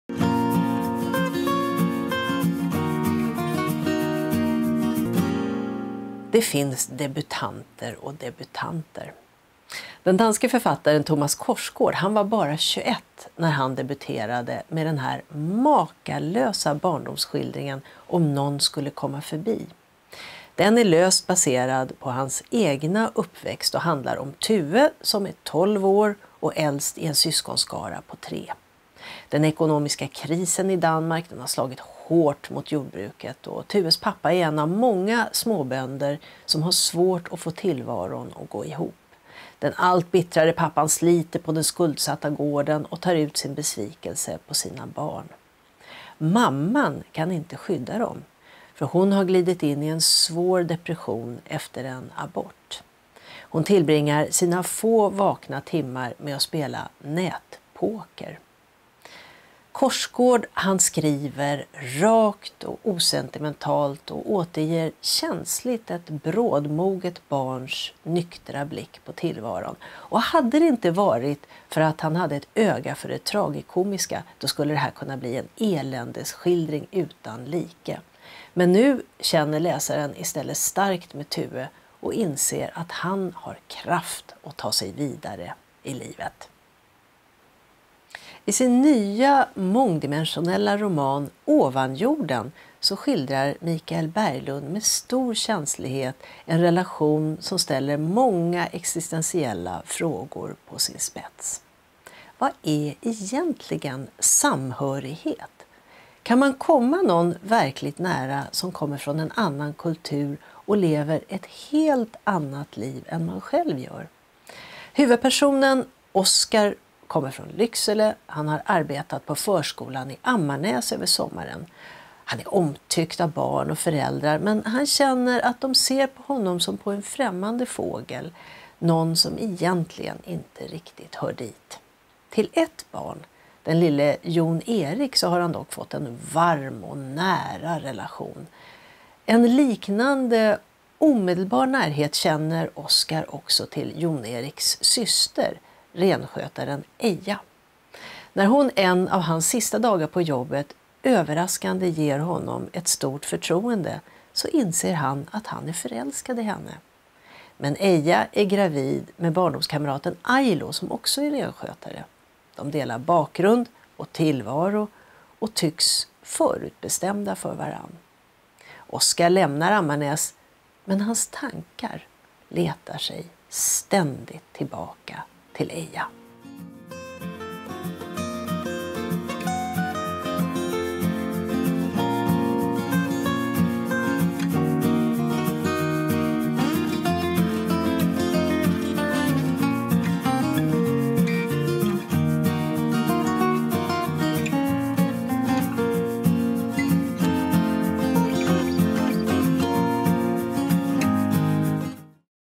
Läsupplevelser som dröjer sig kvar!
Det finns böcker som är härliga att läsa i stunden, men som inte lämnar några större spår. Men så finns det andra verk, som sjunker in i en och dröjer sig kvar. Som läsare kommer man på sig själv med att återvända in i romanvärlden, tänka handlingen vidare. Här är två sådana romaner.

Om någon skulle komma förbi, Thomas Korsgaard (Weyler, övers Helena Hansson)
Ovanjorden, Mikael Berglund, (Bonniers)